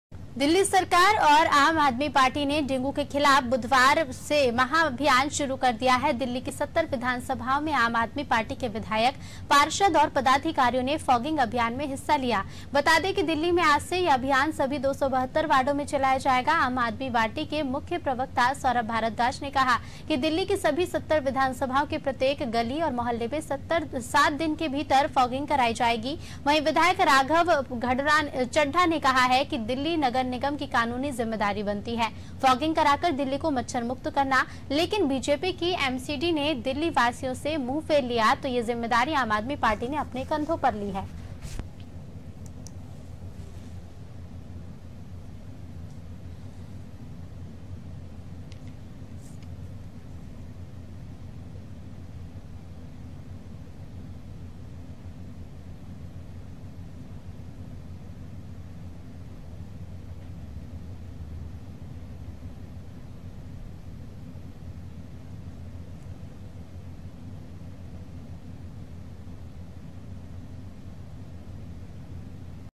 दिल्ली: डेंगू के खिलाफ फॉगिंग महाभियान | Aaj ki Taza Khabar | Anaadi Tv | Hindi News
दिल्ली डिजास्टर मैनेजमेंट के आदेश के बाद दिल्ली सरकार ने एक बड़ा फैसला लेते लिए 1 नवंबर से दिल्ली की सभी सरकारी और प्राइवेट स्कूलों की सभी कक्षाओं को खोलने के आदेश जारी किए है...इस दौरान स्कूलों को कोरोना गाइड लाइन के अनुसार समाजिक दूरी के नियमों का सख्ती से पालन करना होगा…कक्षाएं 50 फीसदी छात्रों के साथ खुल सकेगी और छात्रों को स्कूल बुलाने के लिए पेरेंटस की सहमति आवश्यक होगी...दिल्ली के उप मुख्यमंत्री मनीष सिसोदिया ने कहा है कि कोई भी स्कूल बच्चों को क्लास में आने के लिए बाध्य नहीं कर सकता है...इसके साथ ही स्कूल को सुनिश्चित करना होगा कि पढ़ाई ऑफलाइन और ऑनलाइन चले...सिसोदिया ने कहा कि सभी स्कूलों को ये सुनिश्चित करना होगा कि उनके सभी टीचिंग और नॉन-टीचिंग स्टाफ पूरी तरह से वैक्सीनेटेड हों...सिसोदिया कहा कि दिल्ली के स्कूलों के लगभग 98 फीसदी टीचर ऐसे हैं जिन्हें वैक्सीन की कम से कम 1 डोज़ लग चुकी है...।